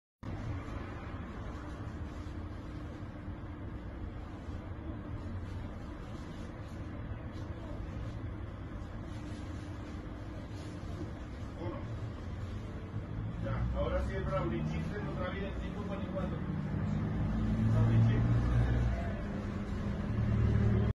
ROCÍA CON GASOLINA A ASALTANTES
Un hombre repostaba tranquilamente su auto en una gasolinera, cuando de pronto llega una panel blanca y tres individuos se bajan para asaltarlo. El hombre no lo duda ni un segundo y los rocía con gasolina, provocando su huida.